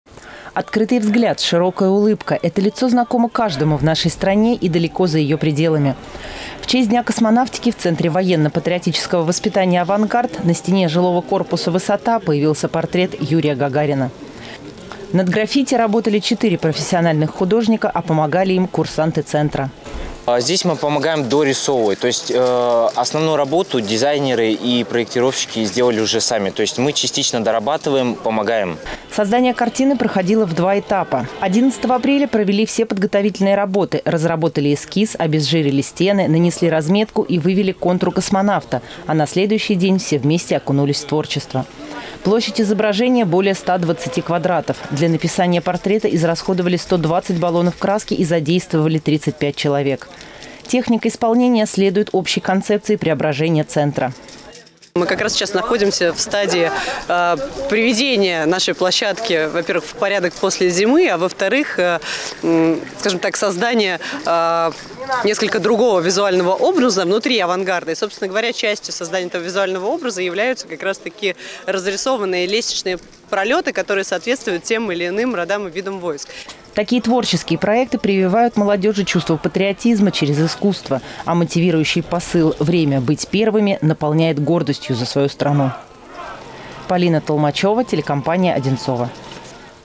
Граффити Гагарина в «Авангарде»
120-метровое граффИти с портретом Героя Советского Союза Юрия Гагарина теперь украшает стену одного из корпусов военно-патриотического центра «Авангард». Изображение нанесли ко Дню космонавтики четыре уличных художника и курсанты центра.